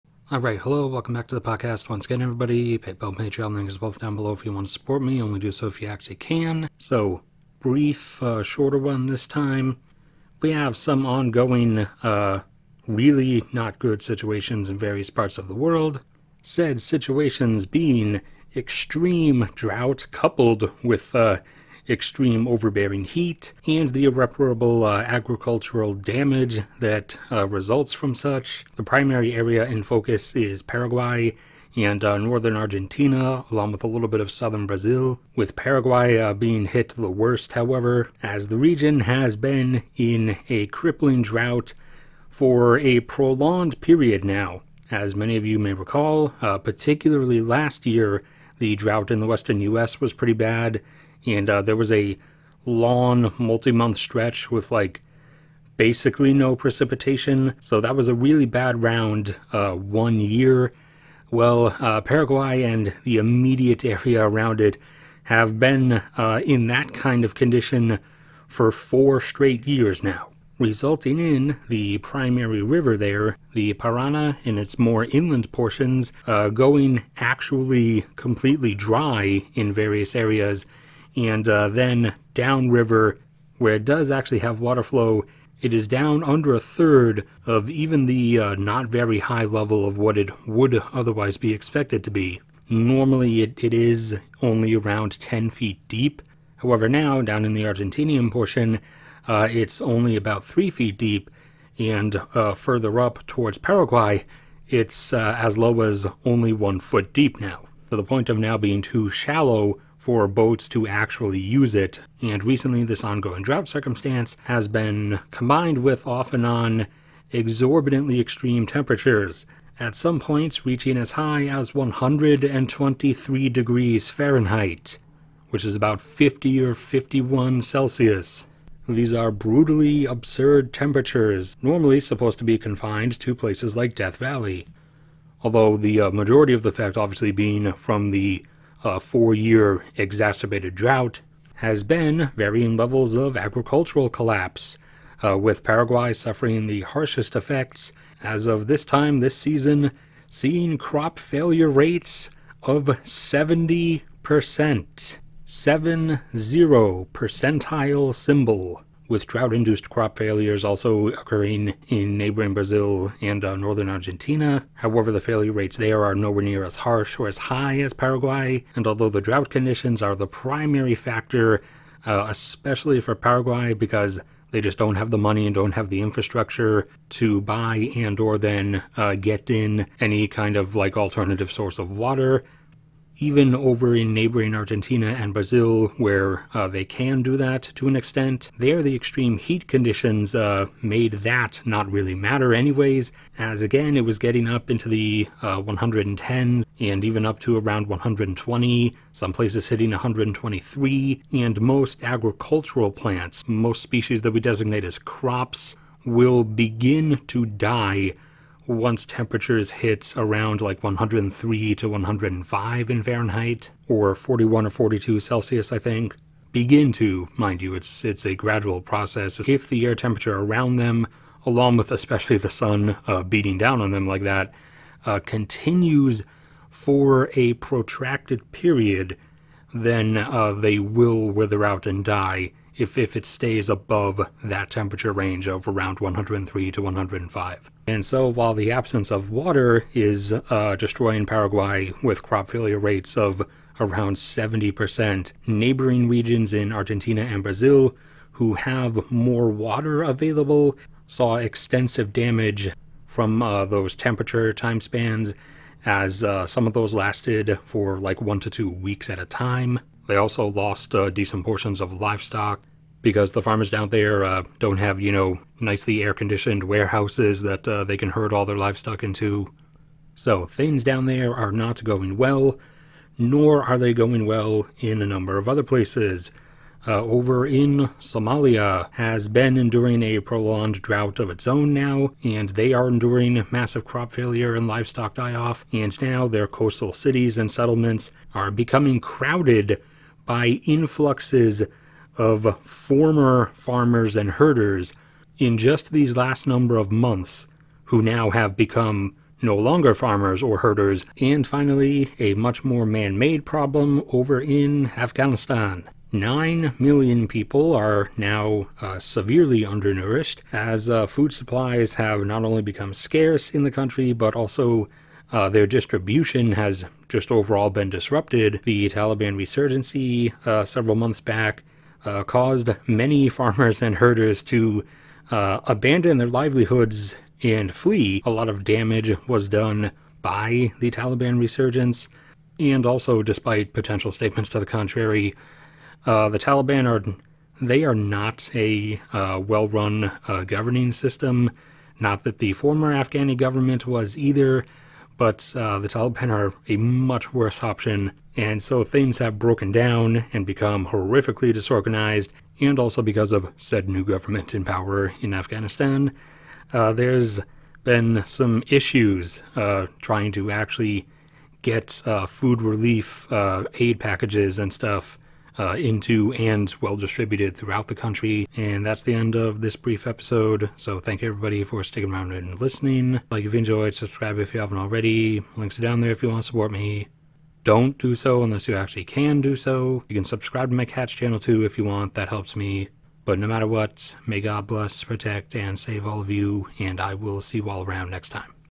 Severe Drought causing Catastrophic Crop Failure in Paraguay
Prolonged severe drought, and extreme heat conditions have brought about crop failure and agricultural damage in Paraguay, Argentina and Brazil.